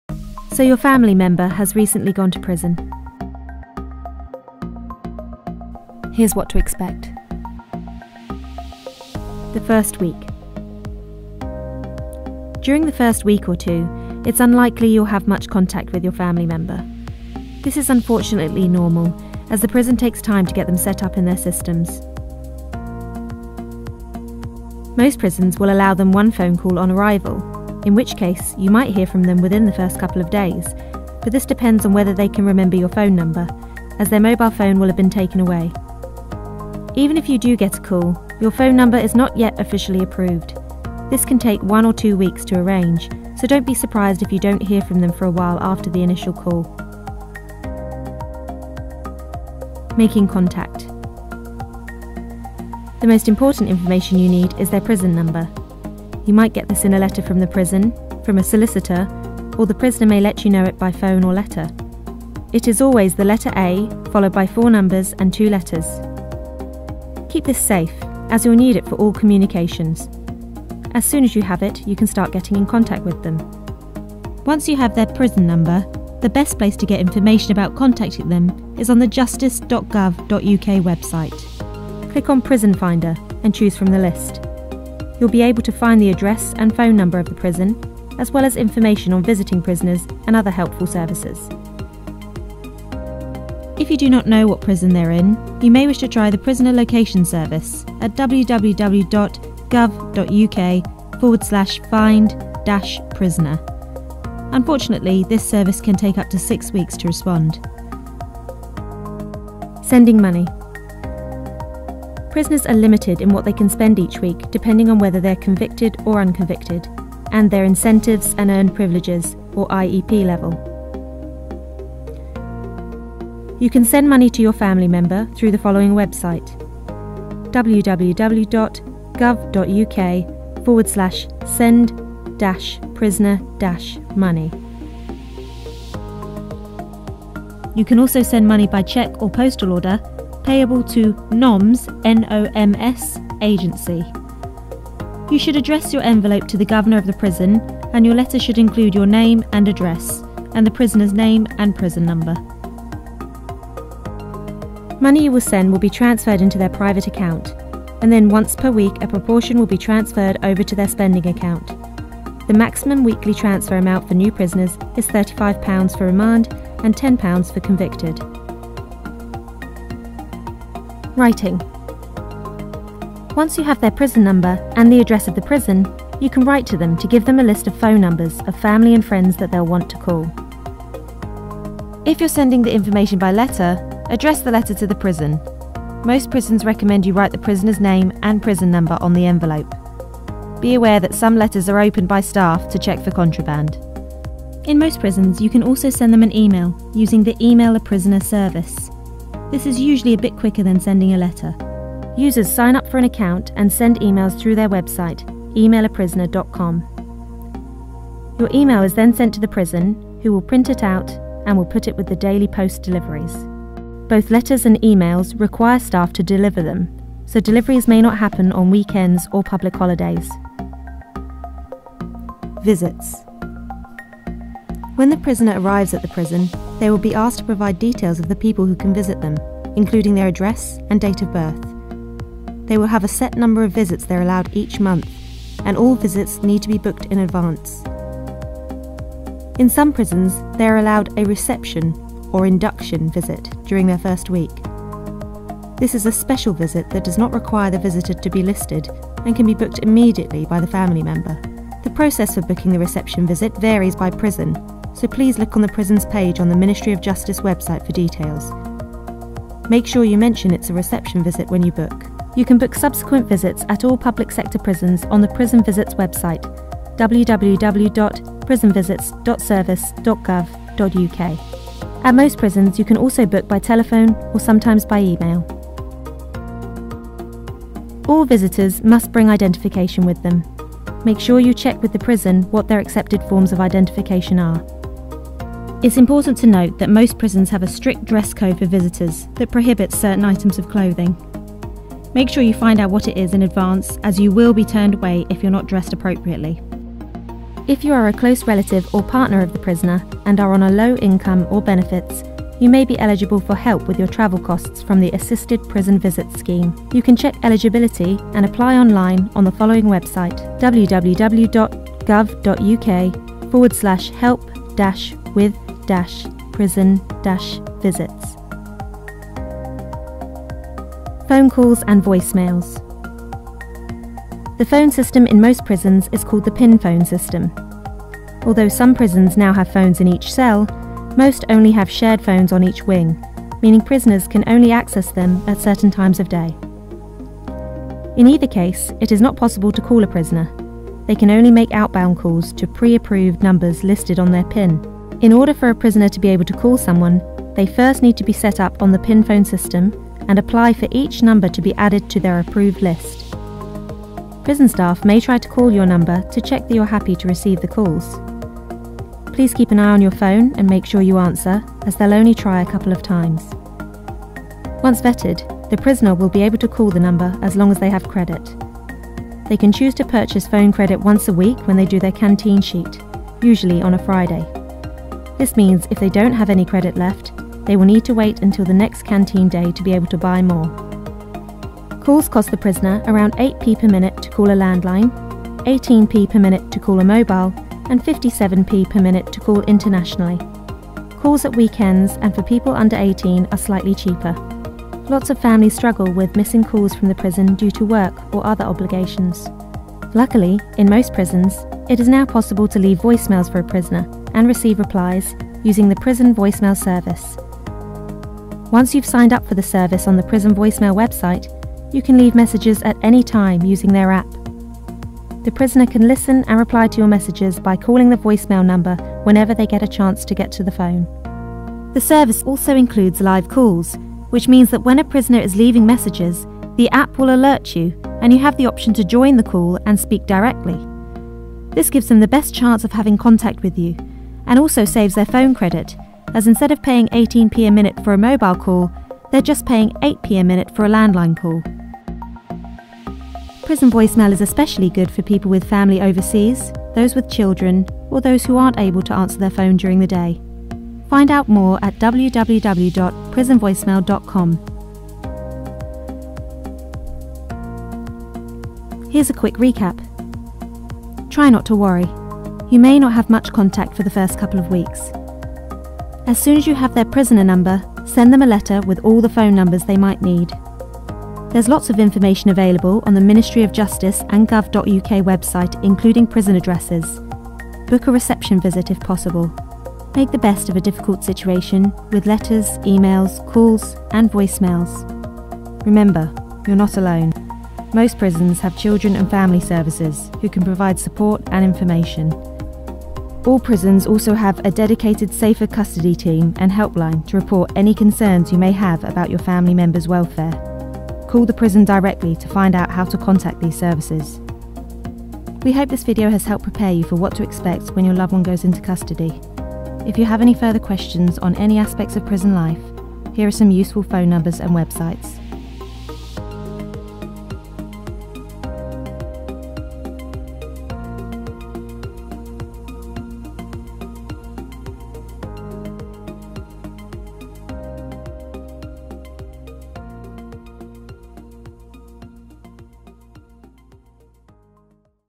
How to contact a prisoner in a UK prison
Having a family member go to prison can be an extremely stressful and confusing time. Knowing what to expect during those difficult first few weeks, and understanding the options available for getting support and communicating, can make the experience a little more bearable.

This video is for the family and friends of people taken into custody in England and Wales. It is regularly updated with new information.

"An excellent resource for families of prisoners and should help alleviate some of the anxiety and worry in the first days and weeks of a loved one’s prison sentence." - Andy Keen-Downs, CEO, Pact

Information last updated August 2018.

Useful websites and numbers:

Find information on a specific prison including address and visiting information

You can send money to your family member through the following

If you are a close relative or partner of the prisoner and are on a low income or benefits, you may be eligible for help with your travel costs from the Assisted Prison Visits Scheme

Send and receive voicemails and reduce prison call costs: prisonvoicemail.com

Prisoners' families helpline: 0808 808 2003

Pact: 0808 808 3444
Prison Voicemail support: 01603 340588
National Information Centre on Children of Offenders nicco.org.uk/